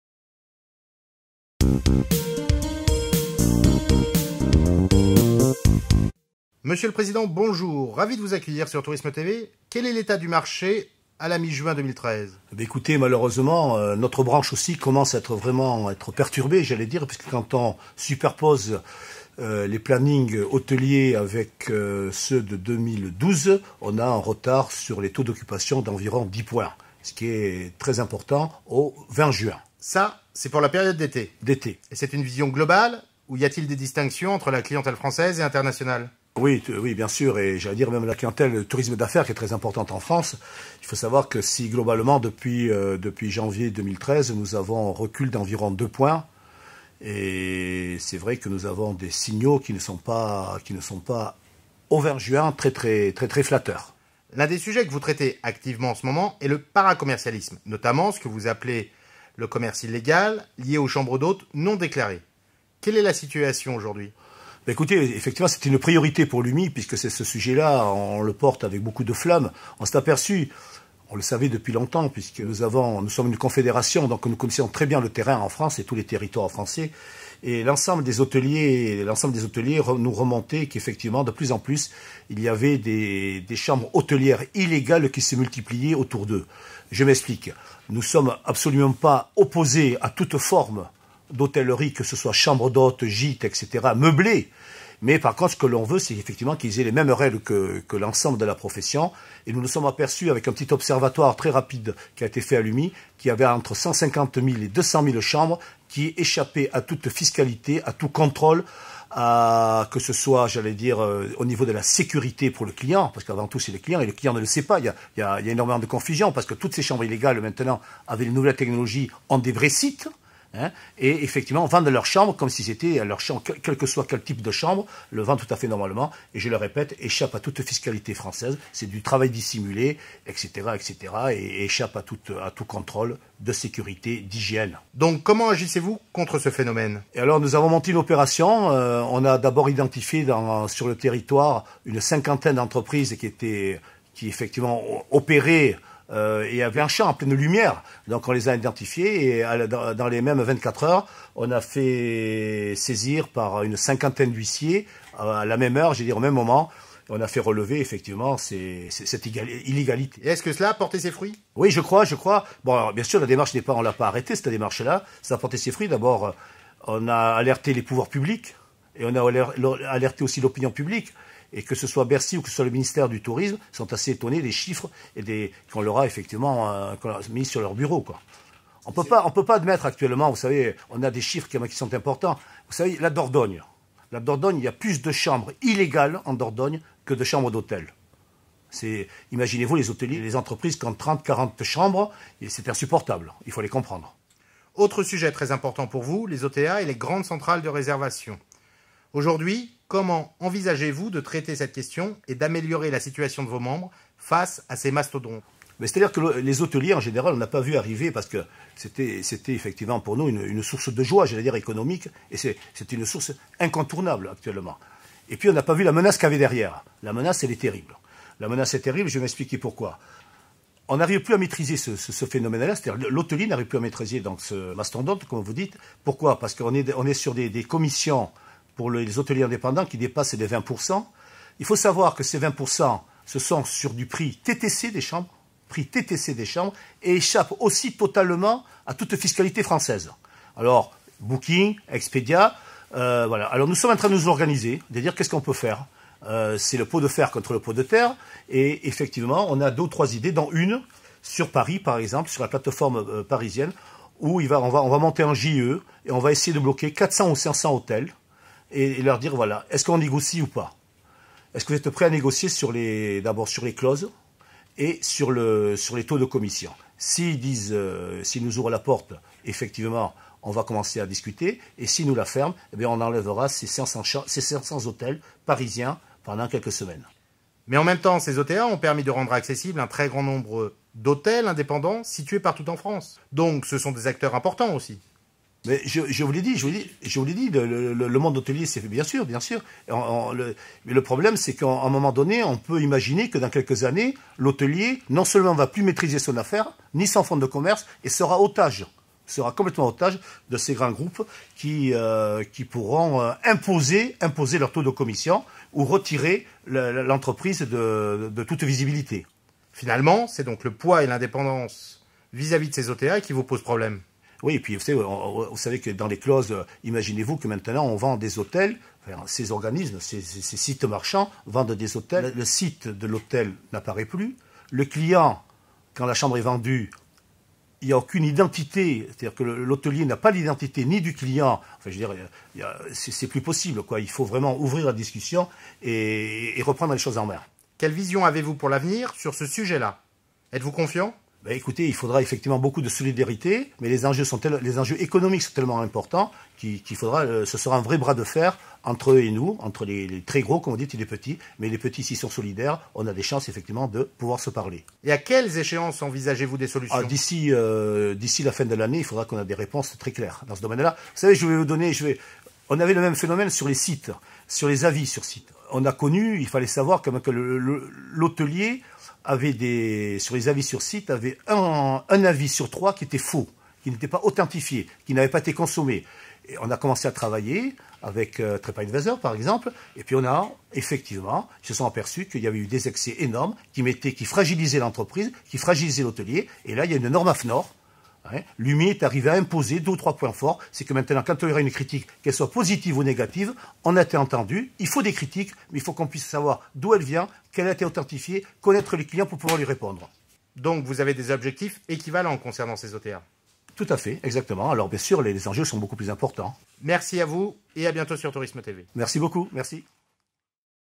ITW de Roland HEGUY, Président confédéral de l'UMIH
Roland Heguy, Président confédéral de l'UMIH (l'Union des Métiers des Industries de l'Hôtellerie) évoque les actions clés de ce syndicat patronal en 2013. Trois éléments clés sur lesquels il est important d'agir, selon Roland Heguy : le phénomène des chambres d'hôtes non déclarées, le poids des centrales de réservation, le rôle des avis de clients sur les sites tels "tripadvisor". Comment l'UMIH agit-elle sur ces trois points, en suivant quels objectifs et à quelles échéances?